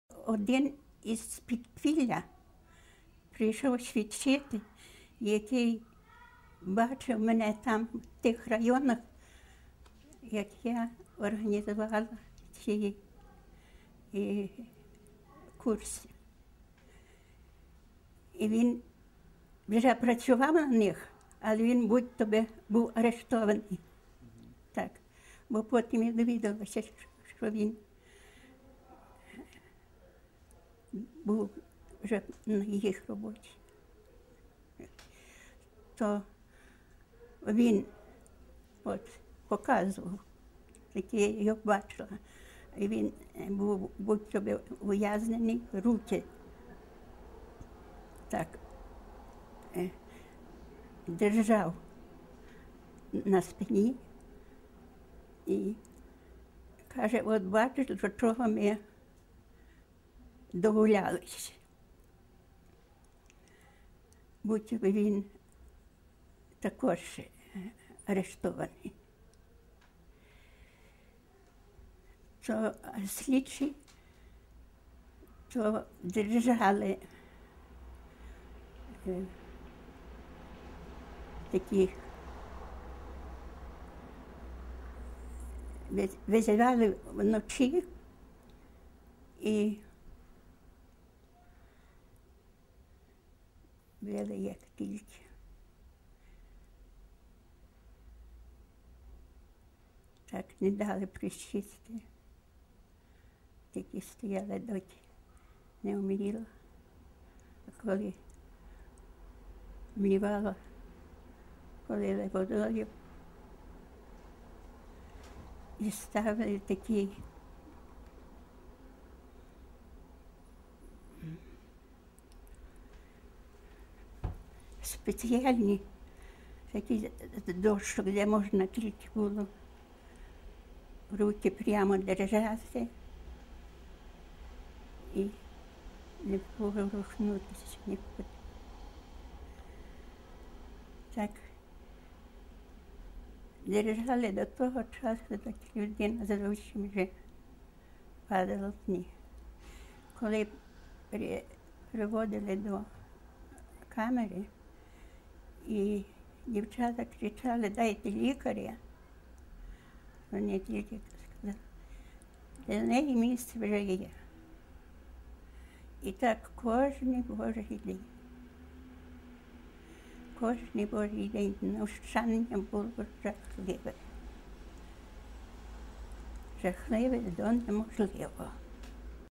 Допити під час слідства в тюрмі м. Івано-Франківськ, 1947 р.
Спогади про "очну ставку" під час допитів у тюрмі м. Івано-Франківськ та про катування під час слідства.
Інтерв'юер -- Тарас Чолій, відеооператор -- Володимир Чопик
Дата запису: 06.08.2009
Установа: Меморіальний музей тоталітарних режимів «Територія Терору»